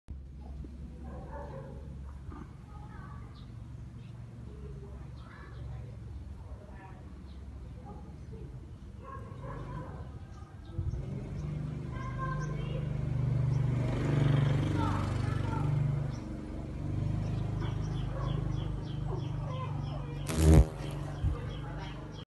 Stink Bug in my garden in Vietnam! insects bugs Vietnam stink bugs
I see a huge variety of interesting creatures whilst trekking in the local mountains, rice paddies and forests here in Vietnam. However, sometimes see something here in my garden - in this case a Vietnamese Lychee Stink Bug. Sorry for the poor quality!